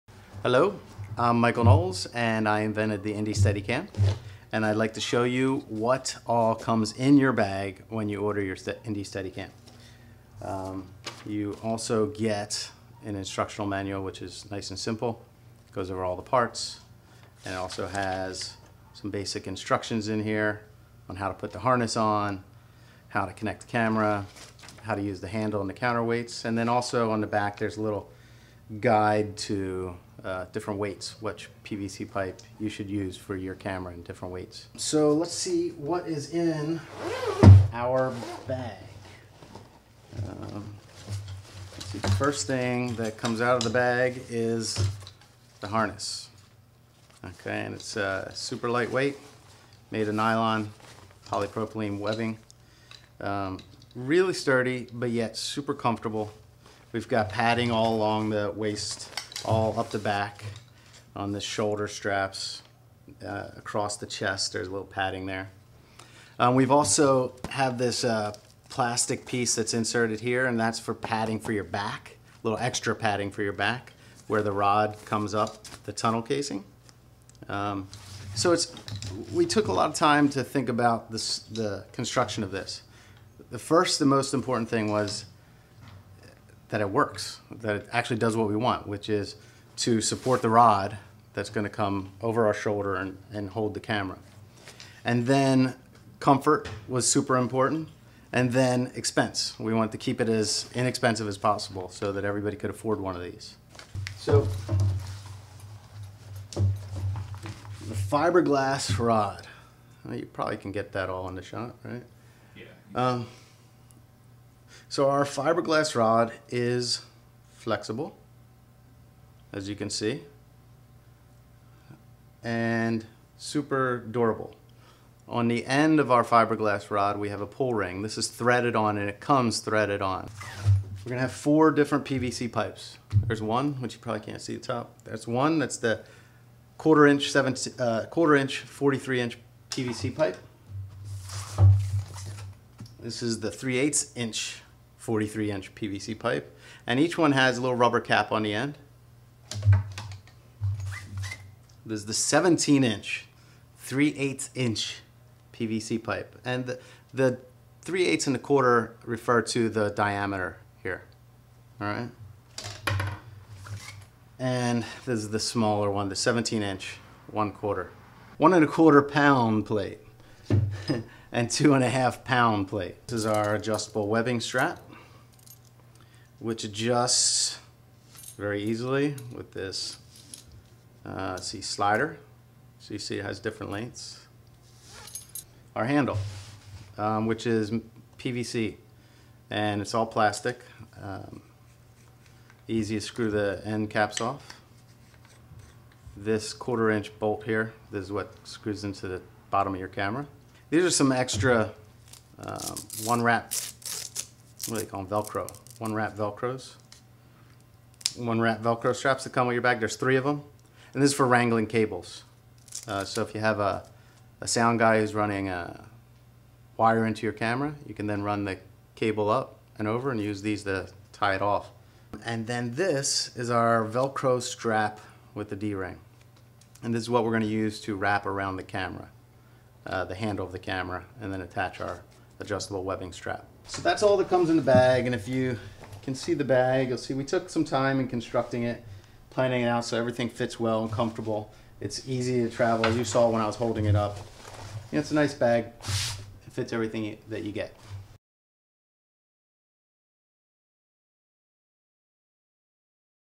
Indy Steadycam - what's in the bag?
a brief description of the items that come with each standard Indy Steadycam package